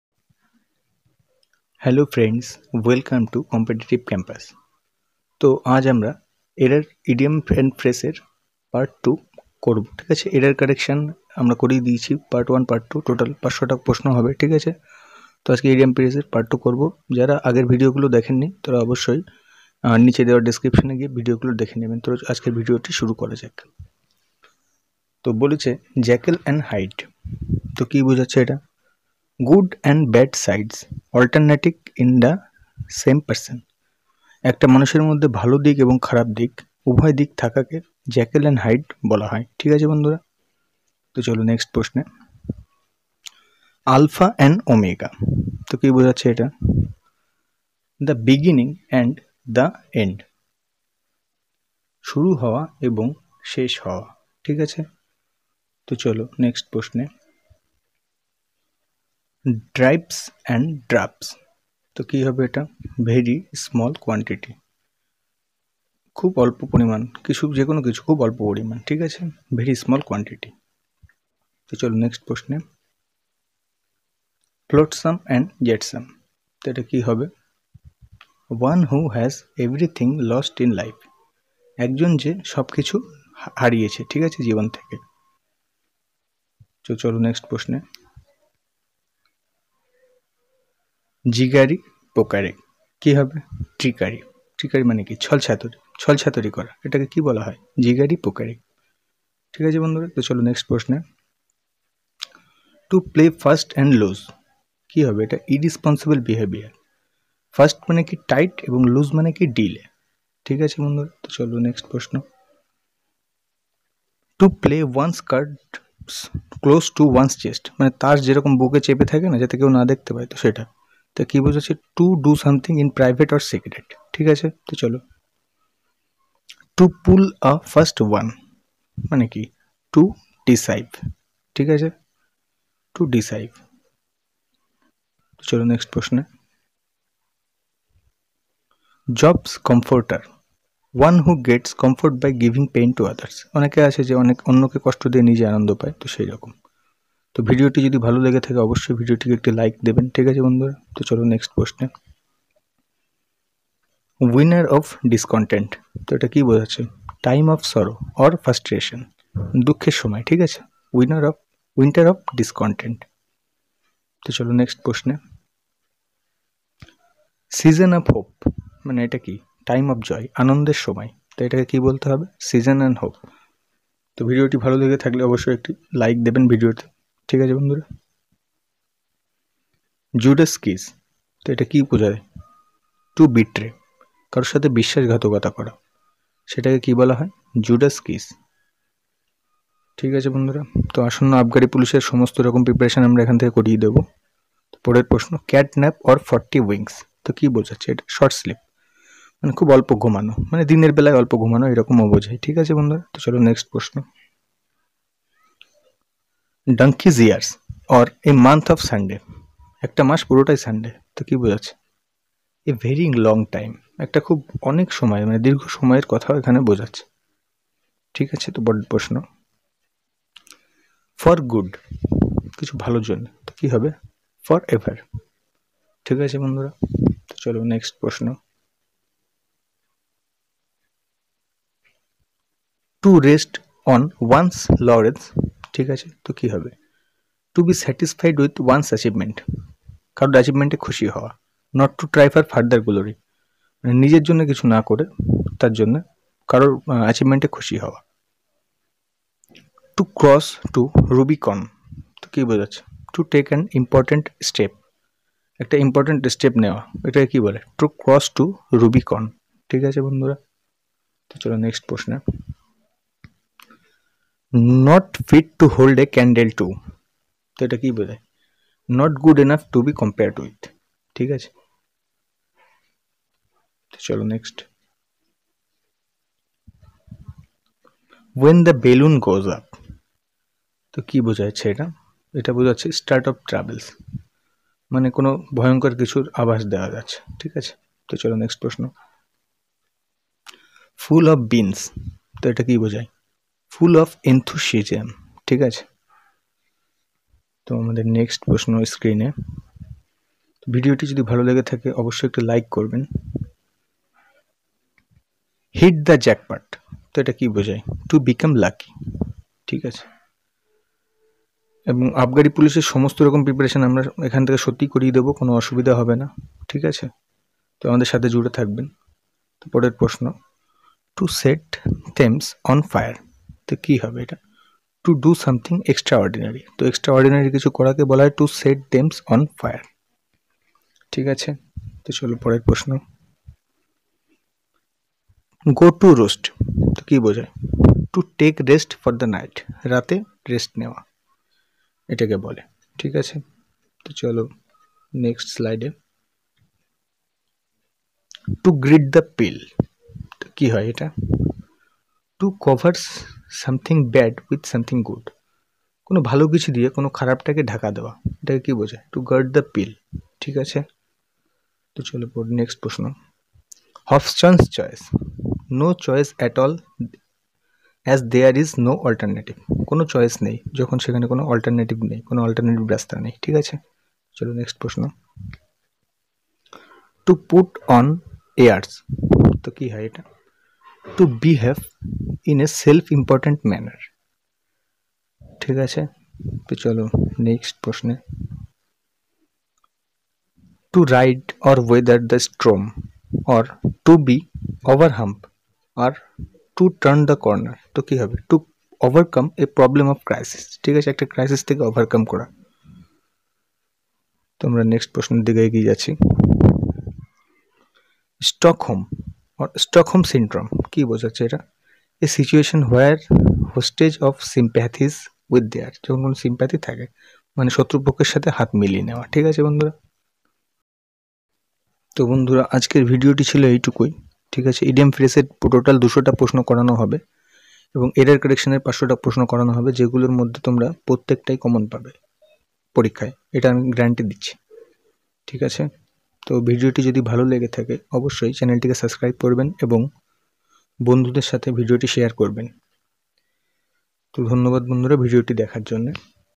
Idiom and phrases | English class for wbp main| Excise Constable English|English competitive campus
Hello friends welcome to our channel today we discuss about important idioms and phrases It will support you to pass the exercise or Abgari constables main and help in SSC and many more exams.. if you like our video please subscribe our channel and hit the like icon and share it with your friends. By Competitive campus